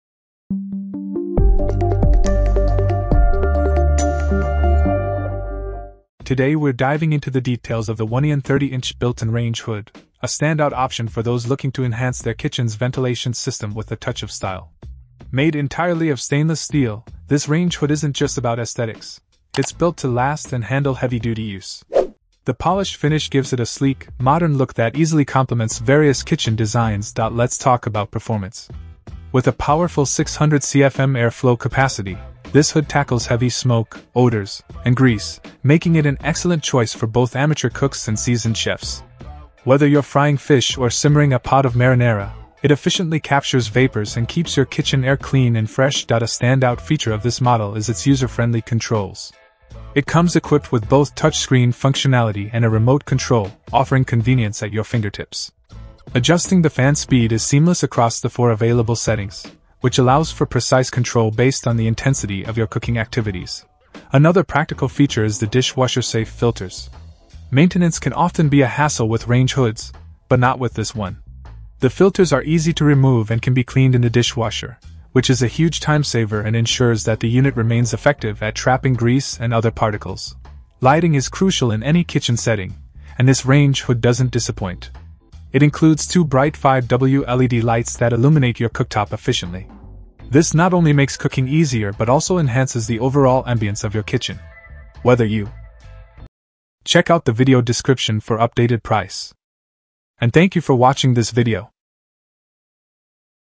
30 Inch Built-in/Insert Range Hood by ONEEON: The Best Upgrade for Your Kitchen Ventilation?
30 Inch Built-in/Insert Range Hood by ONEEON: The Best Upgrade for Your Kitchen Ventilation?

0:00 Intro
0:06 Review

Things that we mentioned in this video:
30 Inch Built-in/Insert Range : B0CSCYTWT2